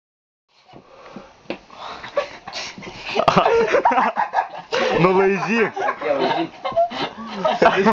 fucking beer
Paulius, stupid funny boy, fucking his beer bottle